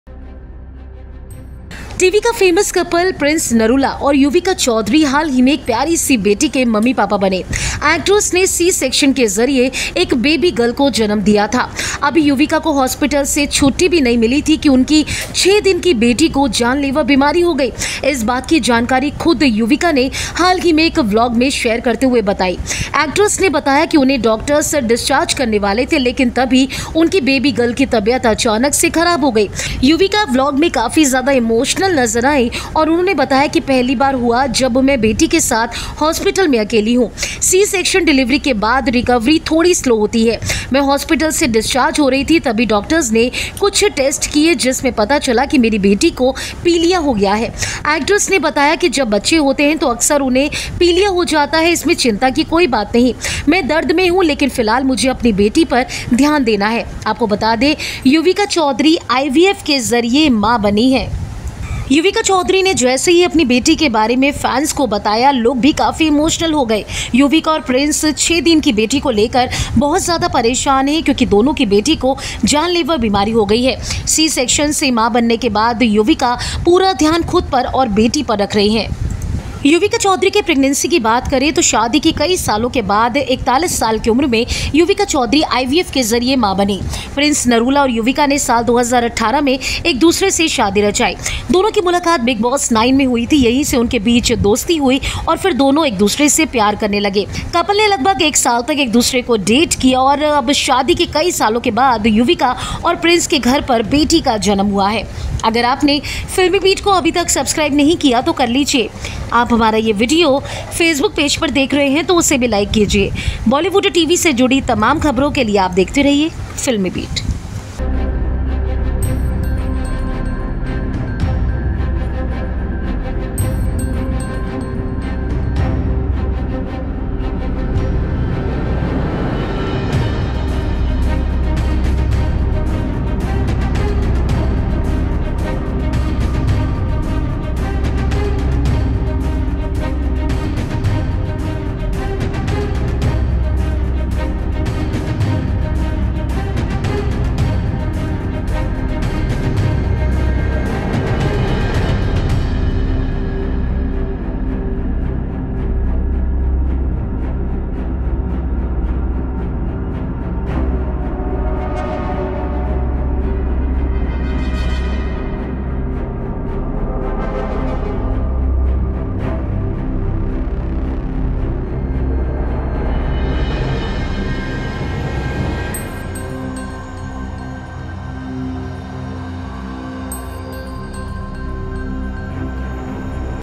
Prince Narula-Yuvika Chaudhary की 6 दिन की बेटी को हुई गंभीर बीमारी, Hospital में है Admit|FilmiBeat
Prince Narula Wife Yuvika ChaudharyReveals She Had C-Section Delivery, Shares her Newborn Baby Girl Got Jaundice.watch Out